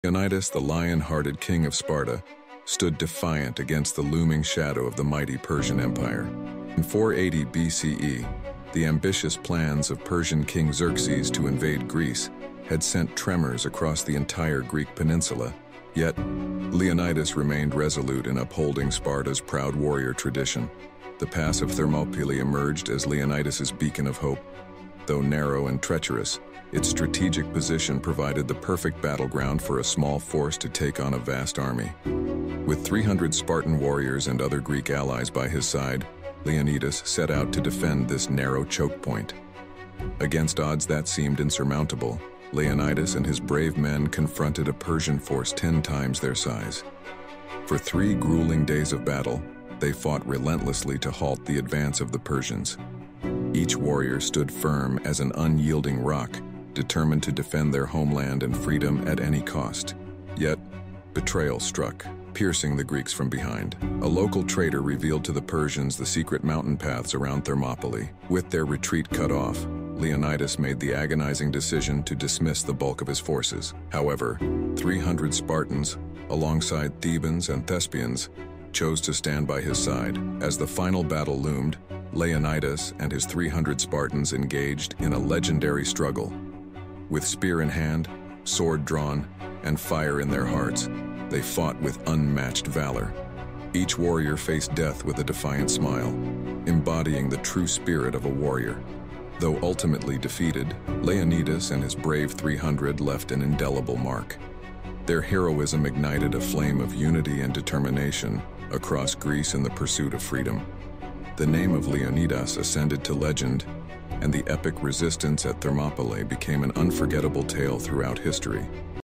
How Did He Become a Legend at Thermopylae? What's Spartan Leonidas' Secret? shortvideo #300
Leonidas, the lion-hearted king of Sparta, stood defiant against the looming shadow of the mighty Persian Empire. In 480 BCE, the ambitious plans of Persian King Xerxes to invade Greece had sent tremors across the entire Greek peninsula. Yet, Leonidas remained resolute in upholding Sparta's proud warrior tradition.

The pass of Thermopylae emerged as Leonidas' beacon of hope. Though narrow and treacherous, its strategic position provided the perfect battleground for a small force to take on a vast army. With 300 Spartan warriors and other Greek allies by his side, Leonidas set out to defend this narrow chokepoint.

Against odds that seemed insurmountable, Leonidas and his brave men confronted a Persian force ten times their size. For three grueling days of battle, they fought relentlessly to halt the advance of the Persians. Each warrior stood firm as an unyielding rock, determined to defend their homeland and freedom at any cost.

Yet, betrayal struck, piercing the Greeks from behind. A local traitor revealed to the Persians the secret mountain paths around Thermopylae. With their retreat cut off, Leonidas made the agonizing decision to dismiss the bulk of his forces. However, 300 Spartans, alongside Thebans and Thespians, chose to stand by his side.

As the final battle loomed, Leonidas and his 300 Spartans engaged in a legendary struggle. With spear in hand, sword drawn, and fire in their hearts, they fought with unmatched valor. Each warrior faced death with a defiant smile, embodying the true spirit of a warrior.

Though ultimately defeated, Leonidas and his brave 300 left an indelible mark. Their heroism ignited a flame of unity and determination across Greece in the pursuit of freedom. The name of Leonidas ascended to legend, and the epic resistance at Thermopylae became an unforgettable tale throughout history.